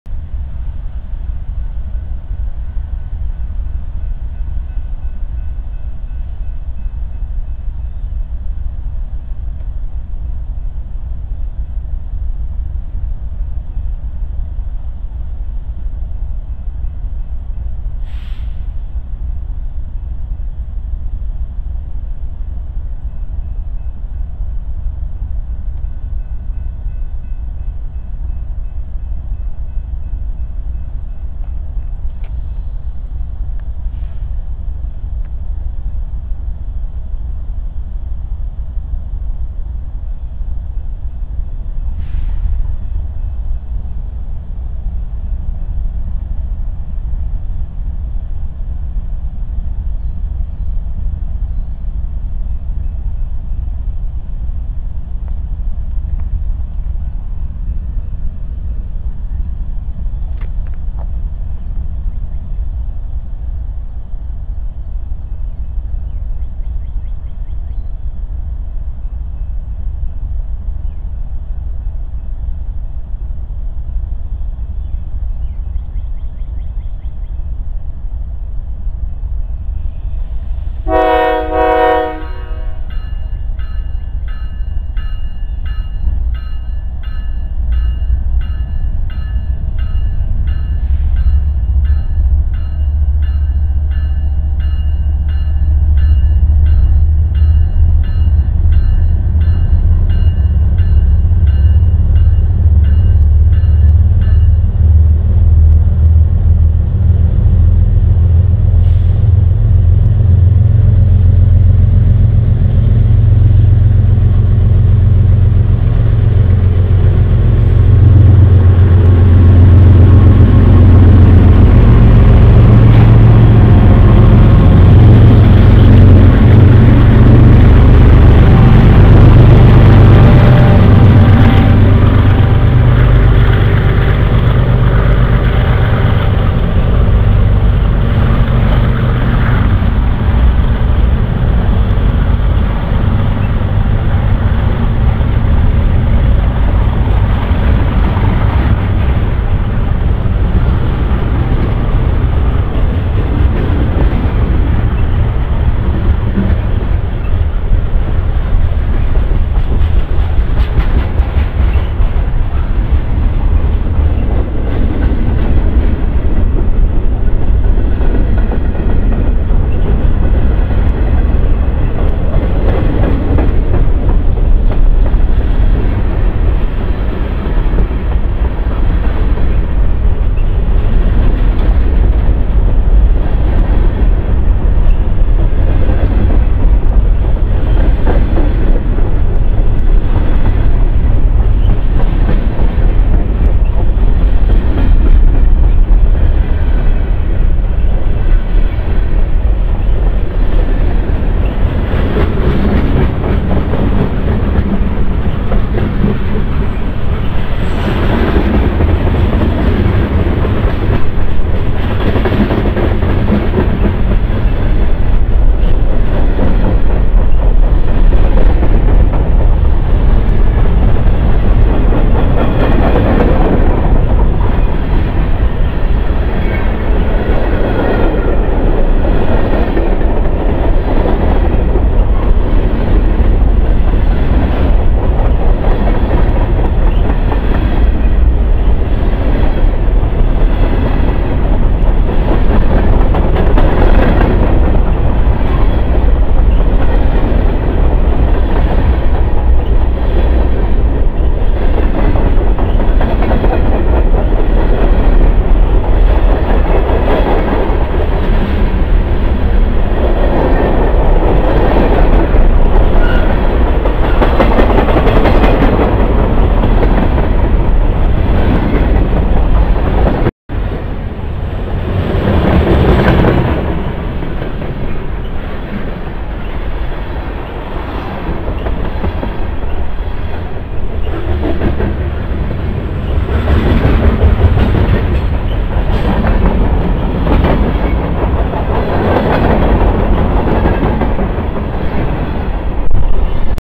VID202007075 BNSF 8291 ES44C4 southbound 07 50 AM 26 July 2020 Edmond Oklahoma by Ronald E Estes
BNSF 8291 ES44C4 southbound 07:50 AM 26-July-2020 Edmond Oklahoma by Ronald E Estes